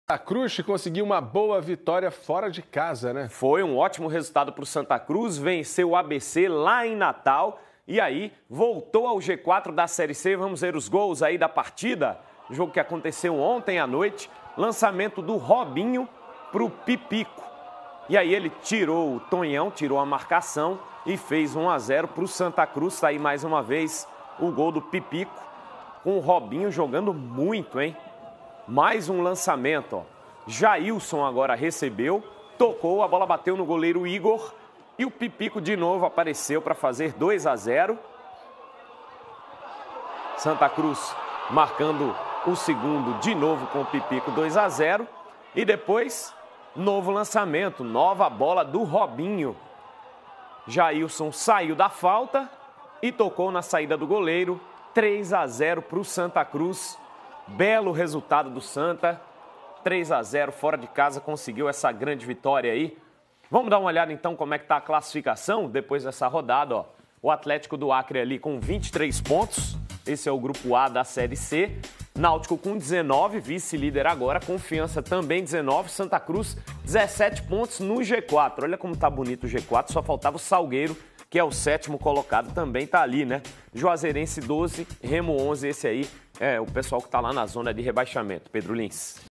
ABC 0 x 3 Santa Cruz - Brasileiro-3ª Divisão 2018
01/07/2018
Campeonato Brasileiro-3ª Divisão - 1ª Fase - A
ABC 0 x 3 Santa Cruz
Gols: Pipico 16 e 22 do 1º; e Jailson 10 do 2º